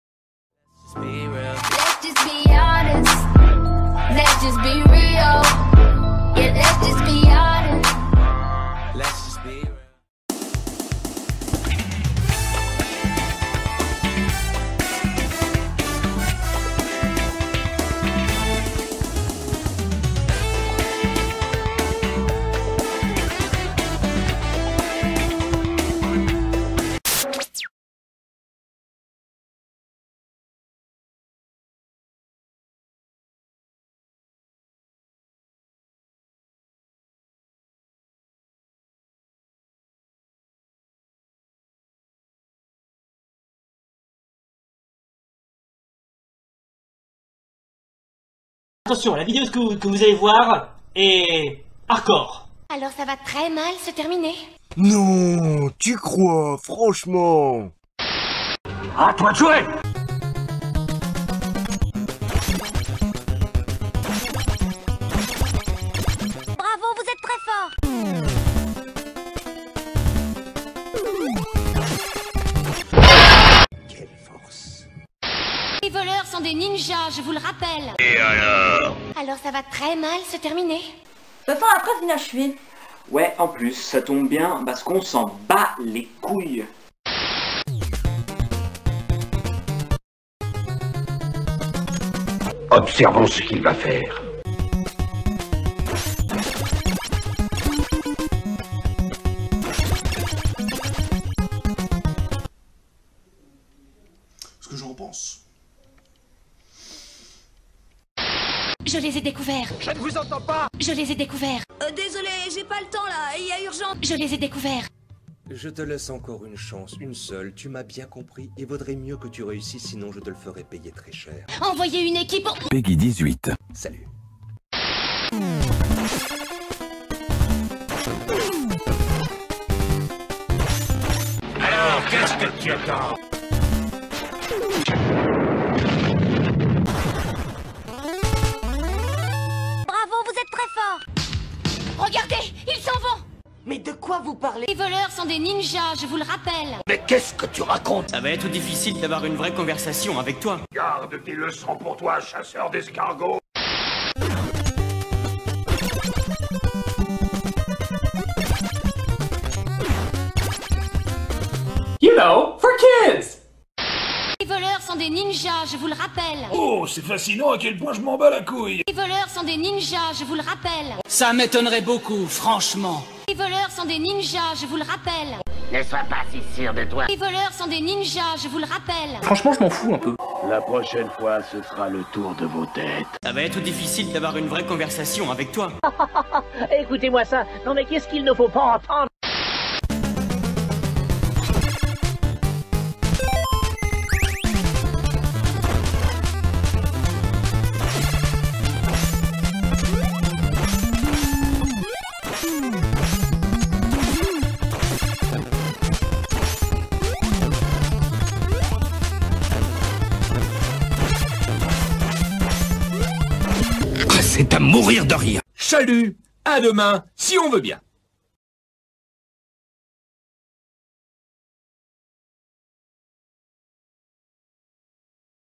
PORC EPIQUE 256, la fois ou j'étais une Tortue Ninja
Nouvelle vidéo parodique sur les Tortues Ninja , alors pour info j'avais déjà mis en ligne cet épisode mais j'ai du le retiré pour ensuite le retravailler voilà bon visionnage ;)